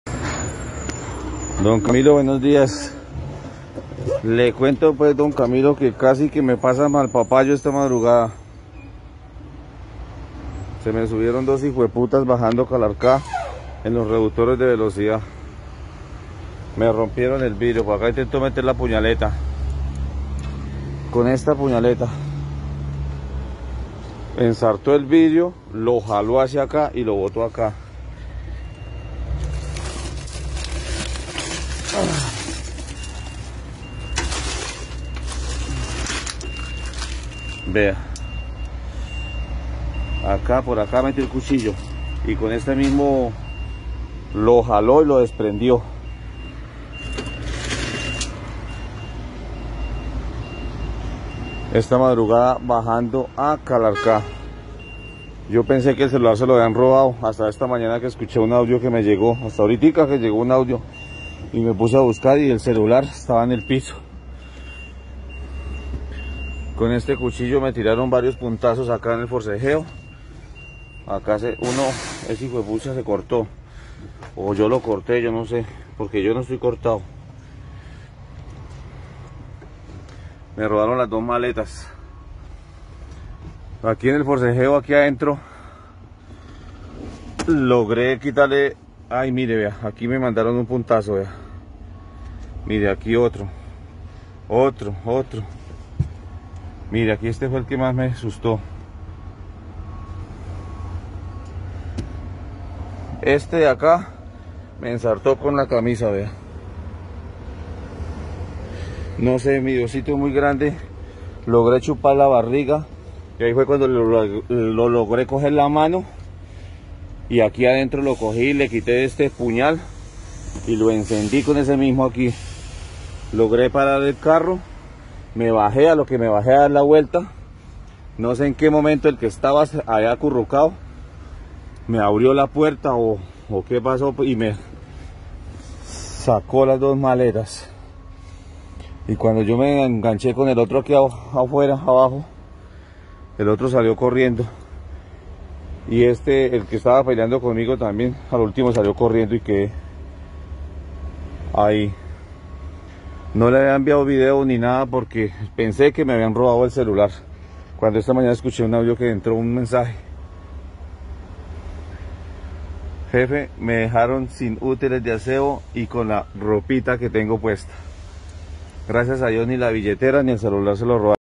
Transportador es atacado por ladrones y se salva de milagro, entre el alto de la línea y Calarca
Inseguridad en el sector del transporte, atacan con arma blanca en el sector de la Línea-Calarca a un transportador.